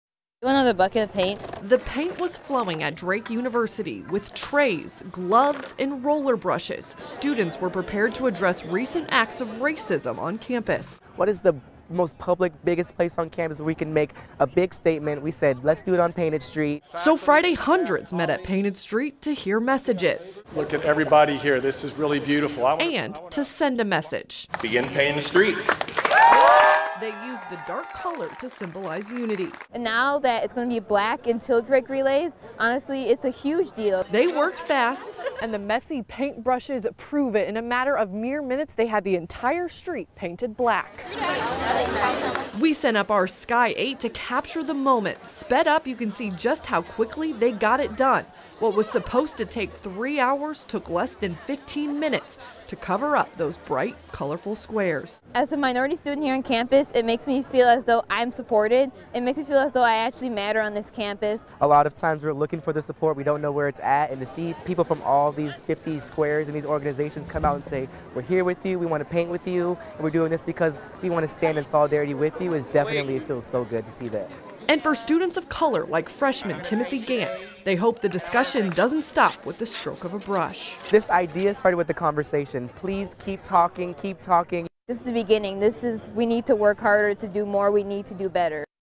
Students paint iconic street to make stand against hate
With trays, gloves and roller brushes, students addressed recent acts of racism by covering Panted Street in black.